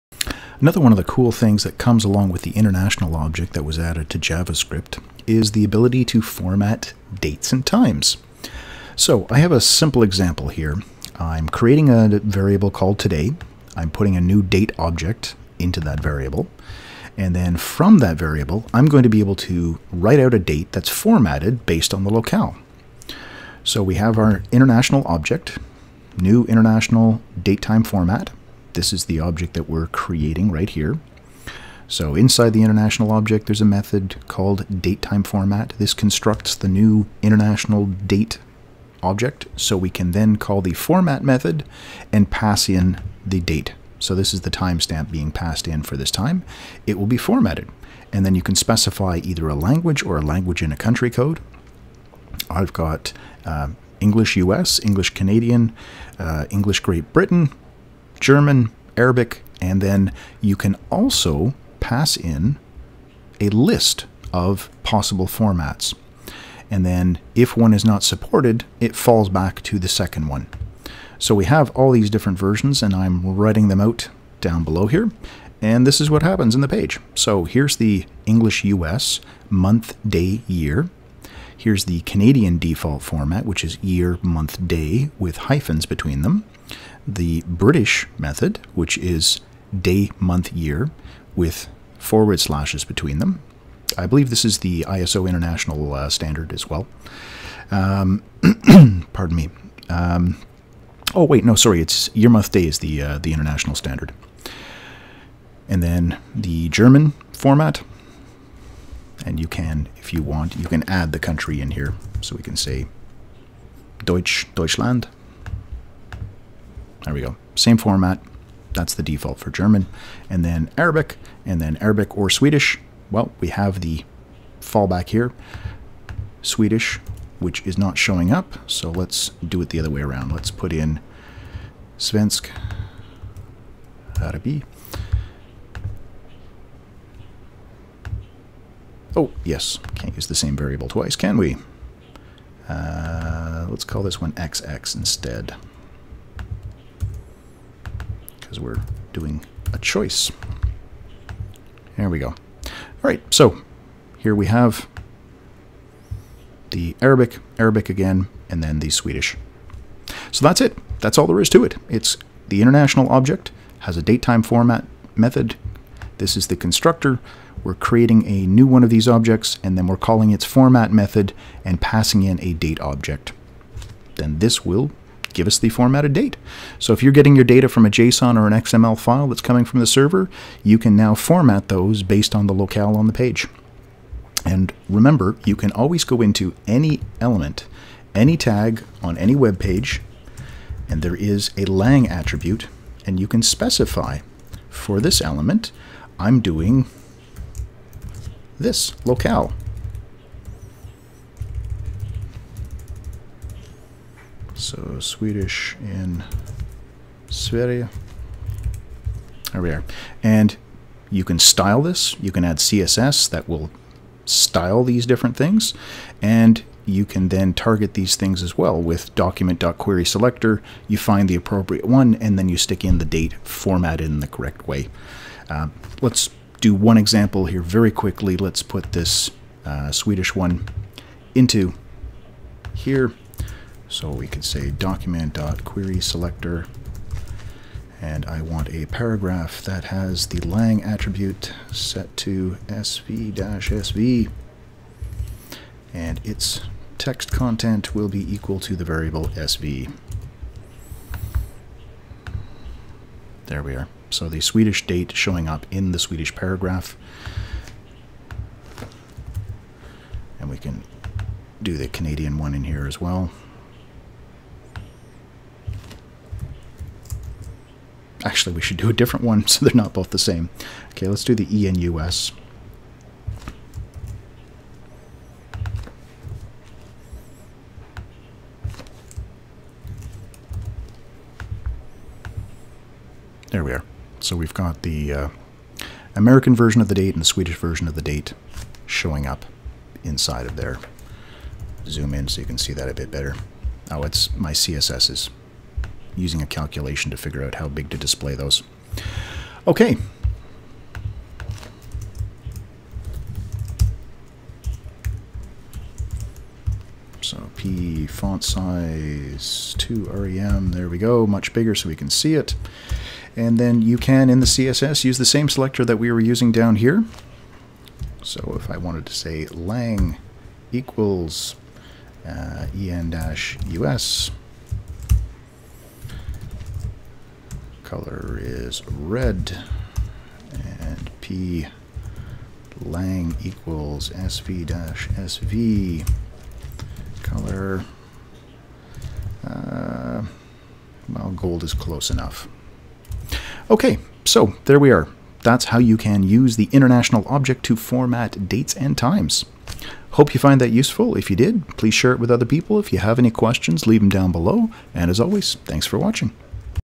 International Date & Time Formatting with JavaScript
This tutorial covers how you can use the new `Intl` object in JavaScript to format and localize date and time values.